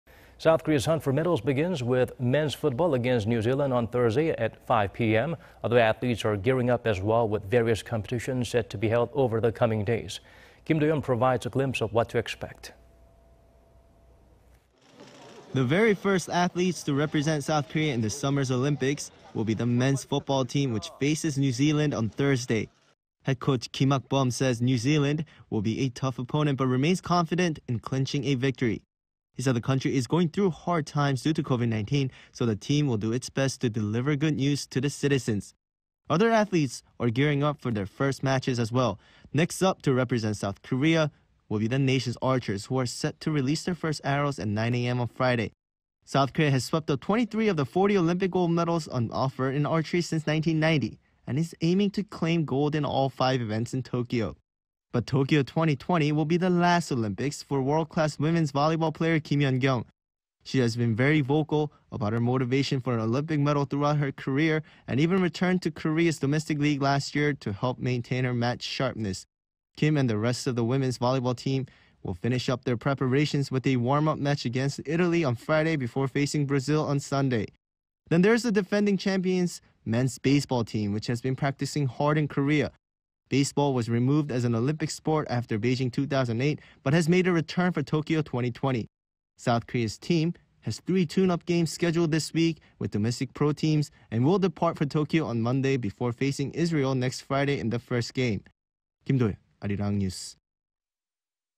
Men's football team first athletes to represent S. Korea at Tokyo 2020, others in final preparations
올림픽 축구 대표팀, 올림픽 선수단 중 첫 게임... 양궁은 다음

South Korea's hunt for medals begins with men's football against New Zealand on Thursday at 5 PM.
Other athletes are gearing up as well with various competitions set to be held over the coming days.
Kim Do-yeon provides a glimpse of what to expect.
The very first athletes to represent South Korea in this summer's Olympics will be the men's football team which faces New Zealand on Thursday.
Head coach, Kim Hak-bum says New Zealand will be a tough opponent but remains confident in clinching a victory.
He said the country is going through hard times due to COVID-19, so the team will do its best to deliver good news to the citizens.
Other athletes are gearing up for their first matches as well.
Next up to represent South Korea will be nation's archers who are set to release their first arrows at 9 AM on Friday.
South Korea has swept up 23 of the 40 Olympic gold medals on offer in archery since 1990... and is aiming to claim gold in all five events in Tokyo.
But Tokyo 2020 will be the last Olympics for world-class women's volleyball player... Kim Yeon-koung.
She has been very vocal about her motivation for an Olympic medal throughout her career... and even returned to Korea's domestic league last year to help maintain her match sharpness.
Kim and the rest of women's volleyball team will finish up their preparations with a warm-up match against Italy on Friday before facing Brazil on Sunday.
Then there's the defending champions men's baseball team which has been practicing hard in Korea.
Baseball was removed as an Olympic sport after Beijing 2008 but has made a return for Tokyo 2020.
South Korea's team has three tune-up games scheduled this week with domestic pro teams, and will depart for Tokyo on Monday before facing Israel next Friday in the first game.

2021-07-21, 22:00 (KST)